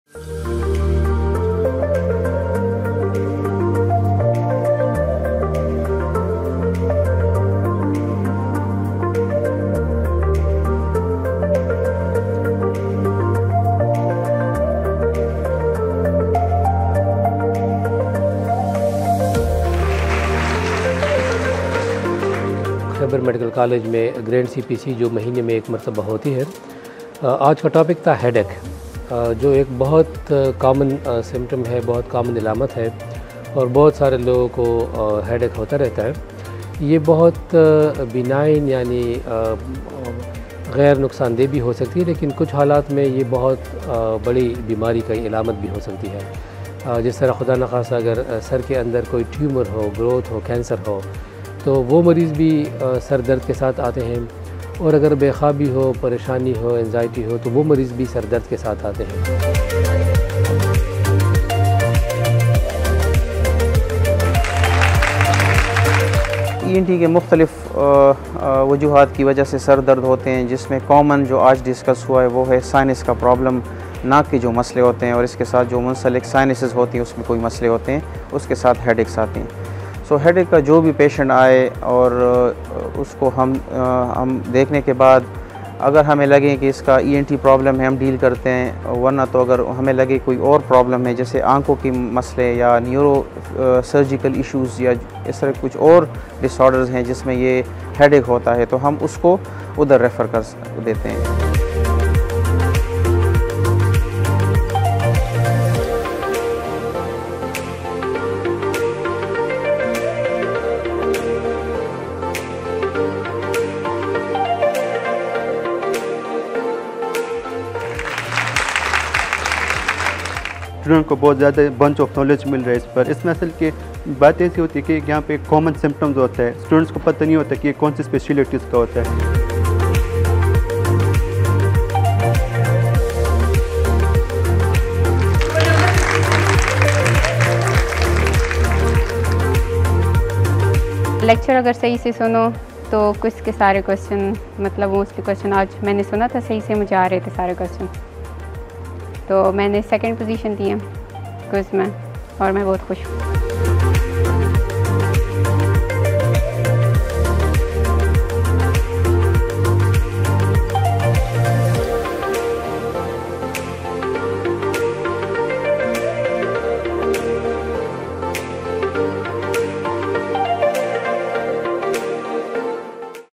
Grand Clinic Pathological Conference on "Headache" | Otorhinolaryngology | MTI-KTH
Grand Clinic Pathological Conference on "Headache" was held in Khyber Medical College on 7th of September, 2022 by Otorhinolaryngology Department under the leadership of Astt. Prof. Dr. Israr ud Din chairman ENT department - KTH.

Prof. Dr. Mahmud Aurangzeb graced the event as chief guest and Prof. Dr. Zakirullah (Ex chairman ENT department KTH) made appearance as a special guest.

Speakers from various Departments, including Associate Prof. Dr. Inam Ullah -Medicine, Asst. Prof. Dr. Bilal Khan - Ophthalmology, Asst. Prof. Dr. Aziz Muhammad - Psychiatry,  Asst. Prof. Dr. Muhammad Hafeez - ENT, Asst. Prof. Dr. Abdul Khaliq - Paediatrics, Asst. Prof. Dr. Irfan Khattak- Neurology and Asst. Prof. Dr. Muhammad Idrees - Neurosurgery gave insights on headaches from the perspective of their related specialties and guided the audience regarding their diagnosis and treatment modalities.

A Quiz competition was held for Final Year MBBS and winners were given prizes by the chief guest.

Over all, it was a fruitful workshop for the audience which aims to treat patients with Headaches using the treatment protocol developed by KTH - renowned consultants.

Follow our Official Social Media pages & get updates on recent happenings & events